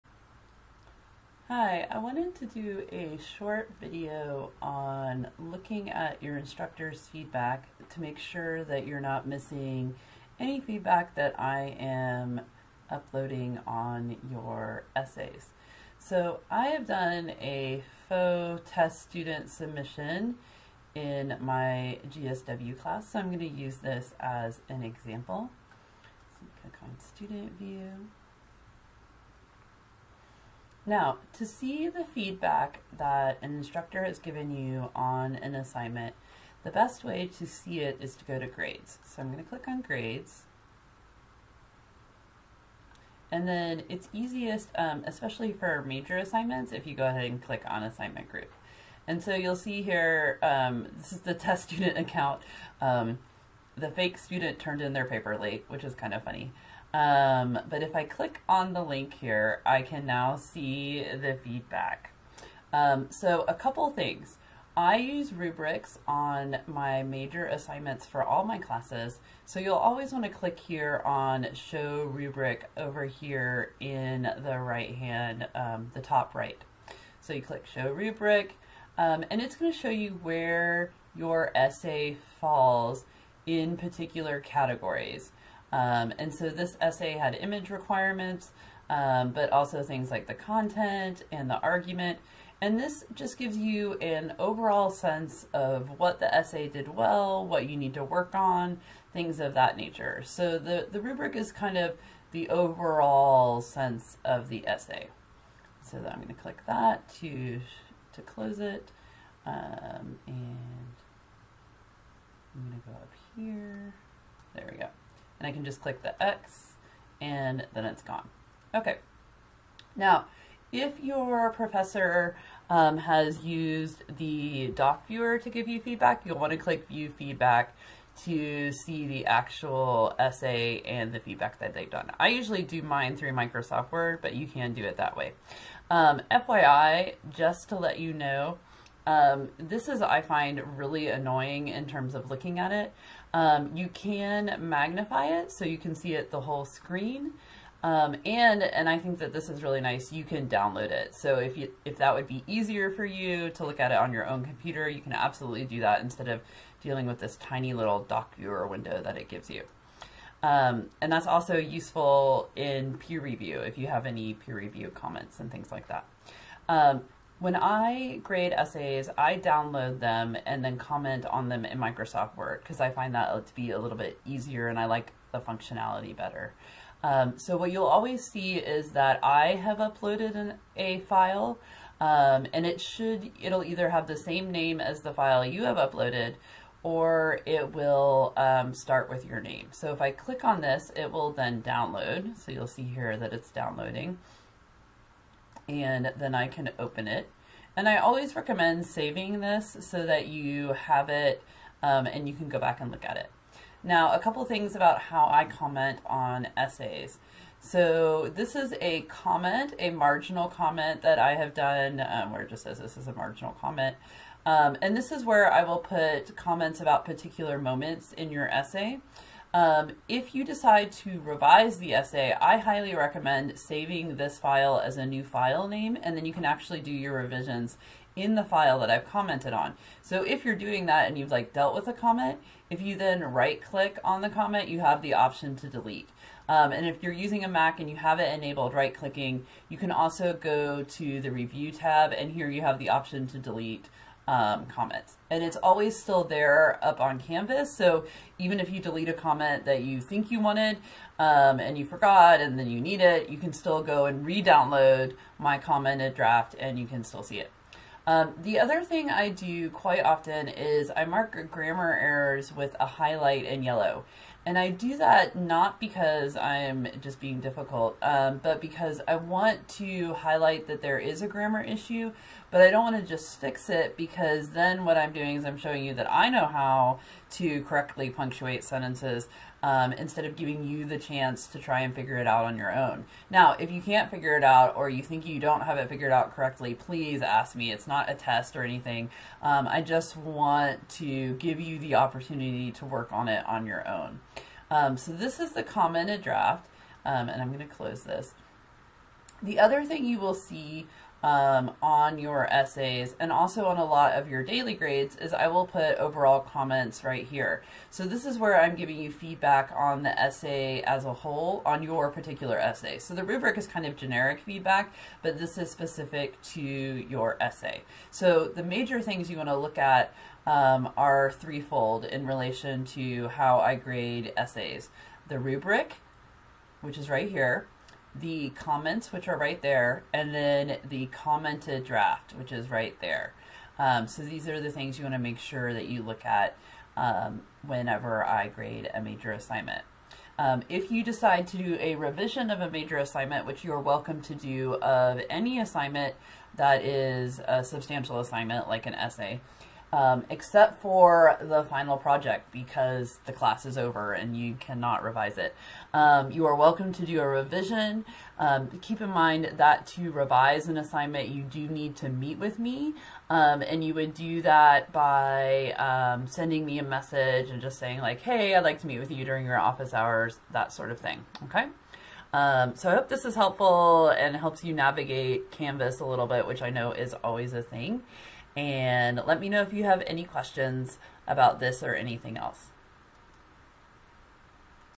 Accessing Instructor Feedback on Essays & Other Major Assignments on Canvas (2022)
Accessing Instructor Feedback on Essays & Other Major Assignments on Canvas